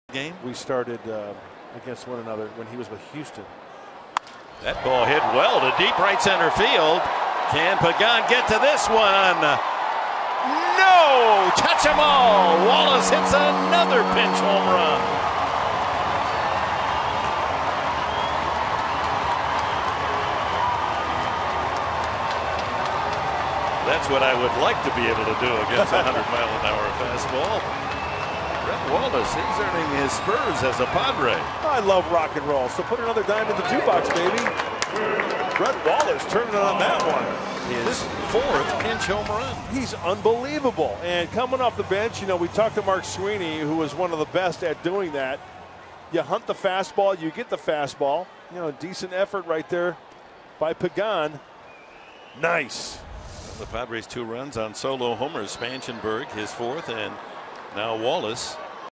SF@SD: Wallace leads off the 7th with homer to center
9/22/15: Brett Wallace greets new reliever Hunter Strickland rudely in the 7th, blasting a solo homer to center to lead off the inning